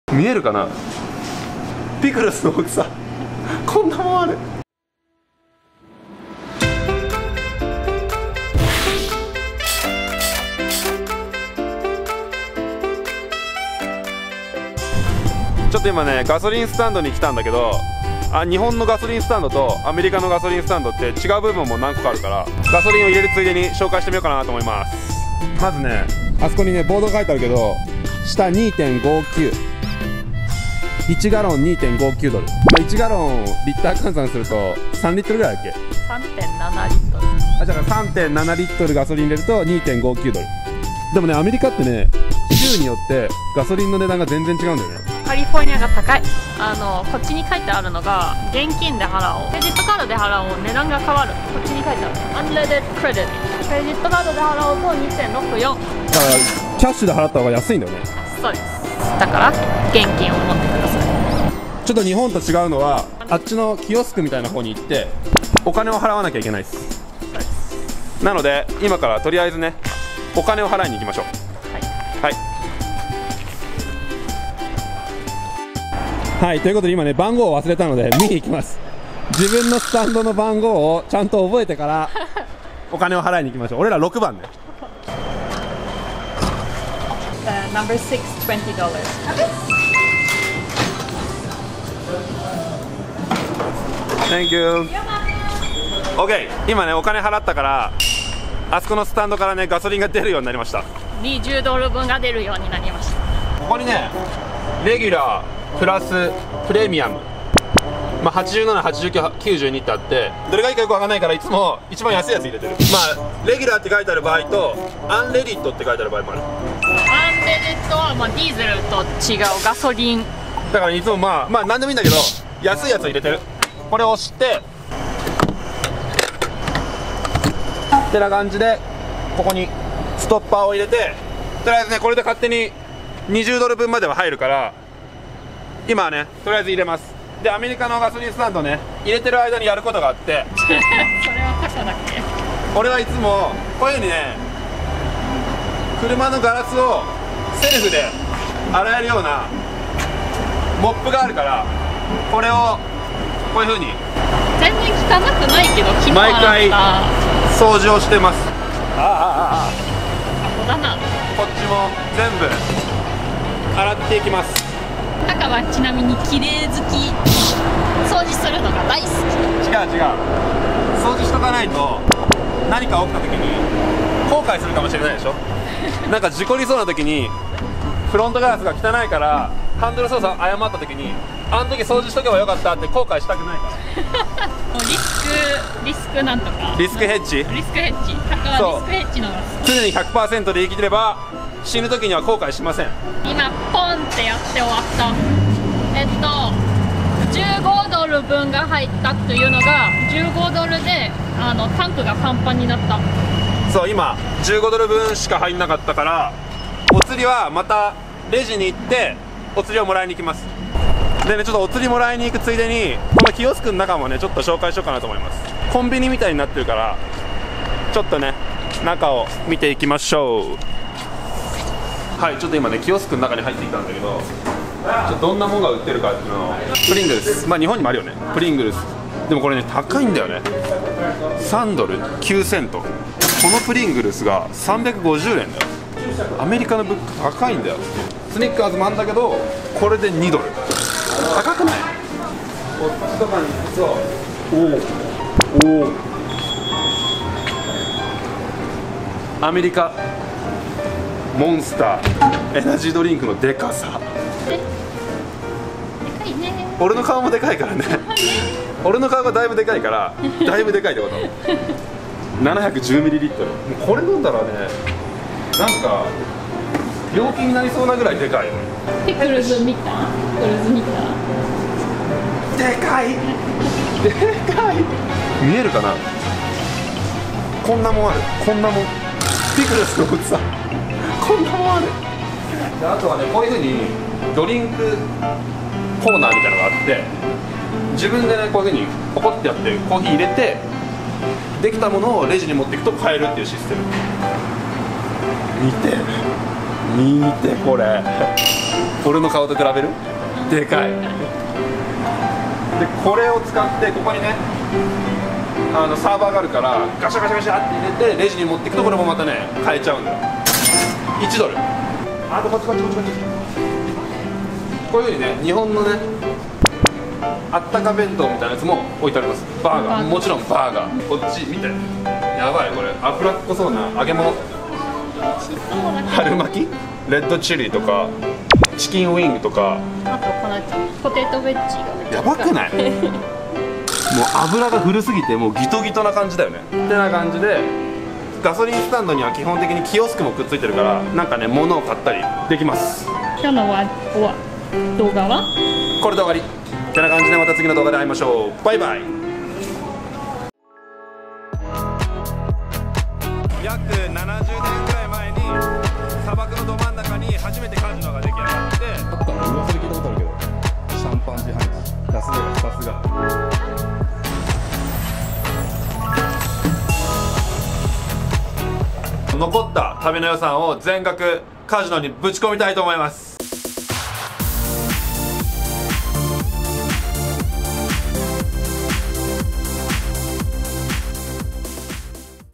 【🇺🇸レンタカー旅】USAのガソリン・スタンド！入れ方＆支払い方法！そしてキオスクをまるごと紹介！【アメリカ西海岸 #08】
アメリカのガソリンスタンドをまるごと紹介！
ガソリンの入れ方＆支払い方法、キオスクなどなど...
日本とアメリカのガソリンスタンドの相違点とは！？